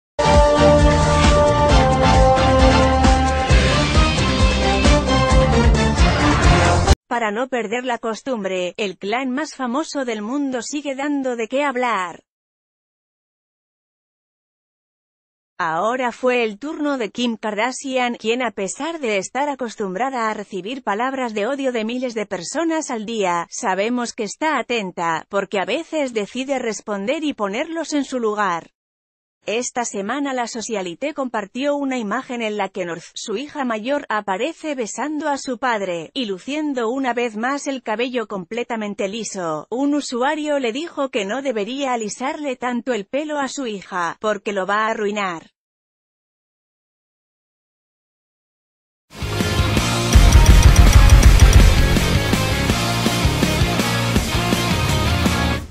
‘Malas madres’, acusan a las Kardashian de no cuidar bien a sus hijos
Para no perder la costumbre, el clan más famoso del mundo sigue dando de qué hablar. Ahora fue el turno de Kim Kardashian, quien a pesar de estar acostumbrada a recibir palabras de odio de miles de personas al día, sabemos que está atenta, porque a veces decide responder y ponerlos en su lugar. Esta...